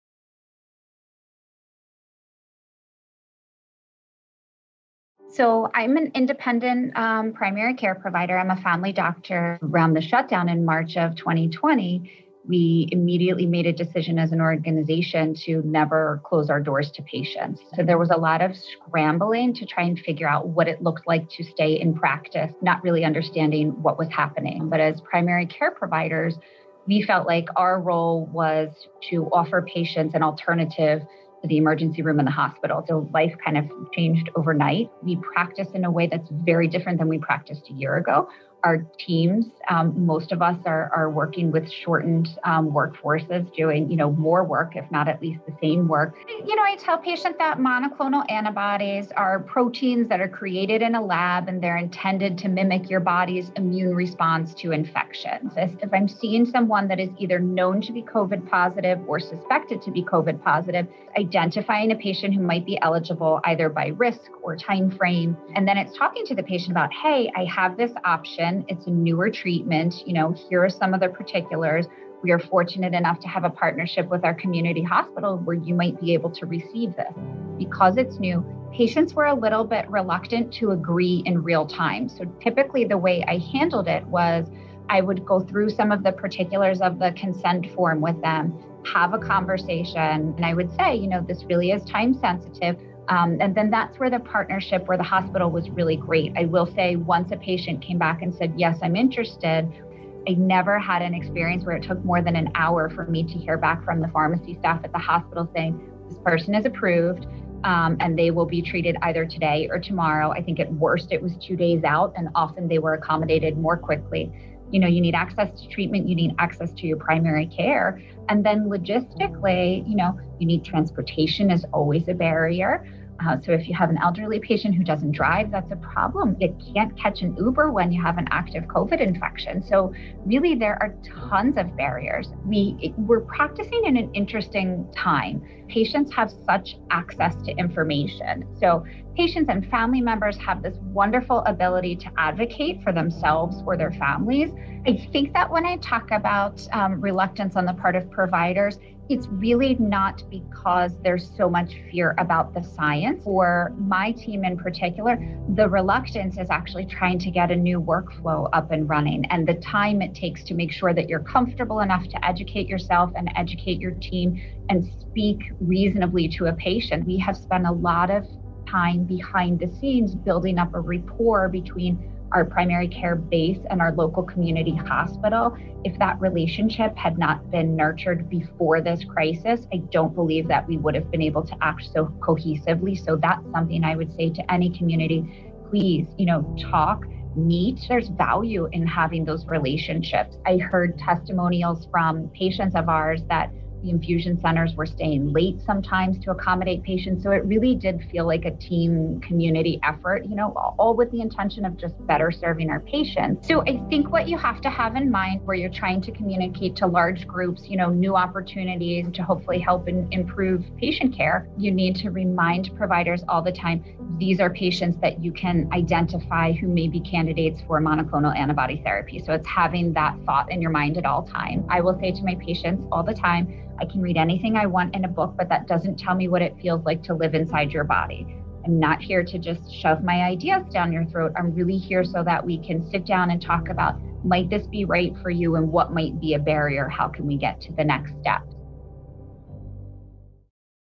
Dr. Natalie McGann: Monoclonal Antibody Treatment in a Primary Practice Setting
Dr. Natalie McGann of TriValley Primary Care discusses how they overcame
obstacles to administer monoclonal antibody treatments to high-risk COVID patients in a primary practice setting.

To find out if you or a loved one may be eligible for monoclonal antibody treatment and to locate a nearby infusion center,